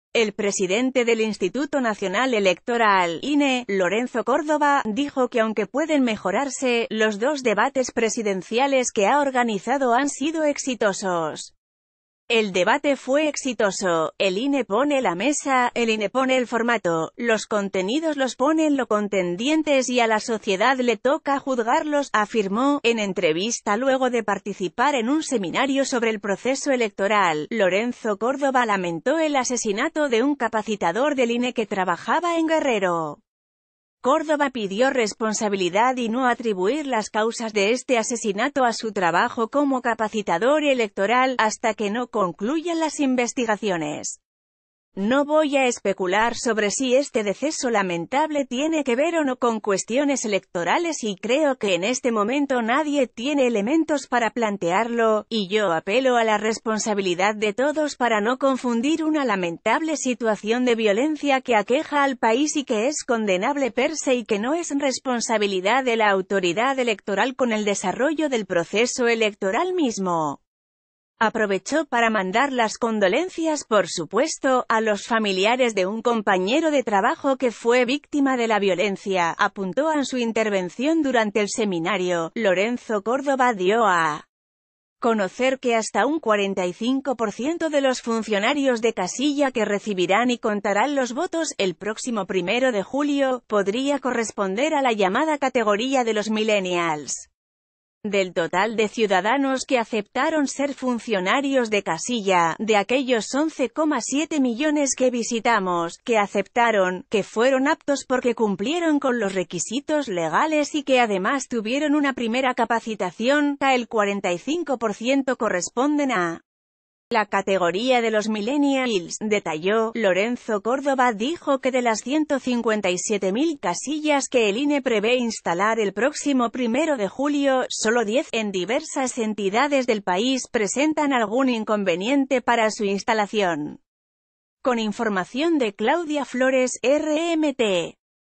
Lorenzo Córdova califica como exitoso el segundo debate presidencial
El presidente del Instituto Nacional Electoral (INE), Lorenzo Córdova, dijo que aunque pueden mejorarse, los dos debates presidenciales que ha organizado han sido exitosos. El debate fue exitoso… El INE pone la mesa, el INE pone el formato, los contenidos los ponen lo contendientes y a la sociedad l...